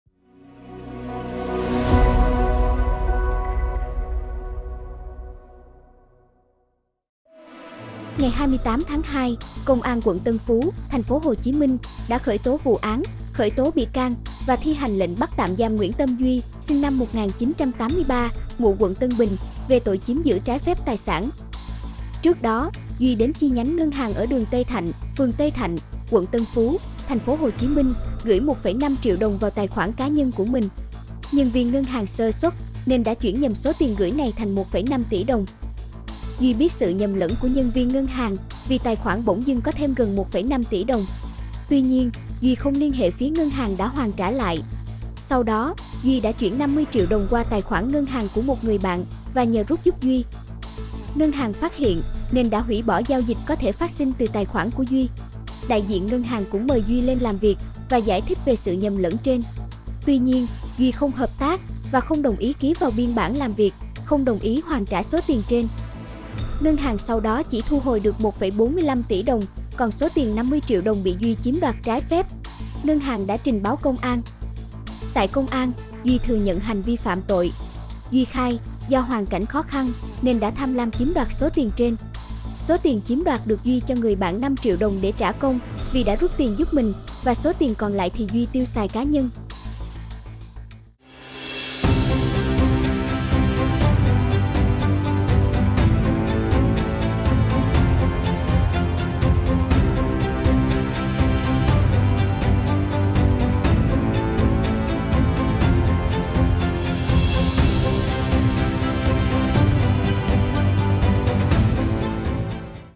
Ngân hàng chuyển nhầm 1,5 triệu đồng thành 1,5 tỷ đồng, người đàn ông tìm cách chiếm đoạt
SGGP | Nguyễn Tâm Duy tới ngân hàng nộp 1,5 triệu đồng vào tài khoản cá nhân nhưng nhân viên ngân hàng chuyển nhầm 1,5 tỷ đồng.